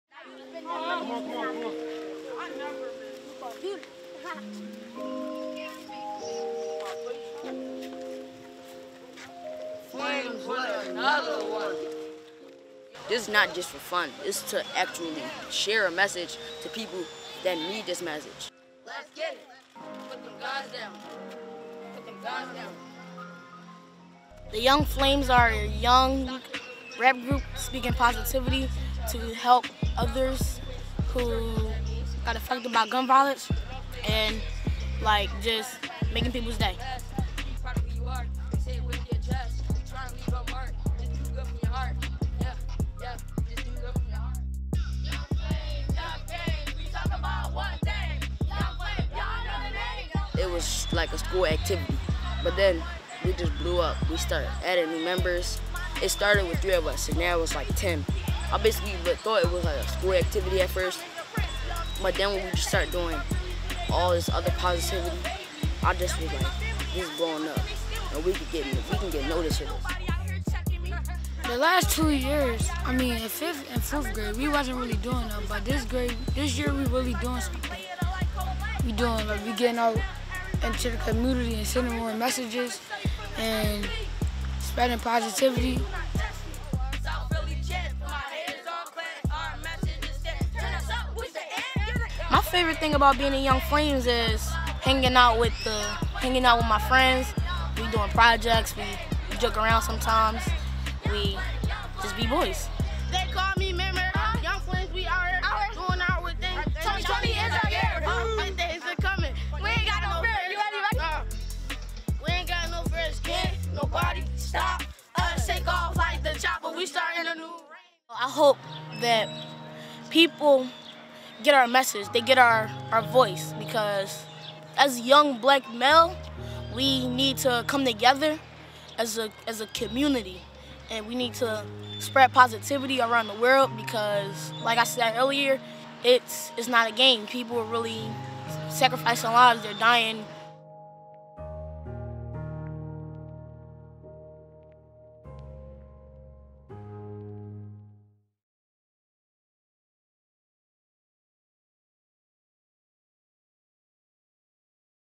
Meet the young Philadelphians using rap to send a message
With songs about racial injustice and ending gun violence, Young Flames is using rap to send a message.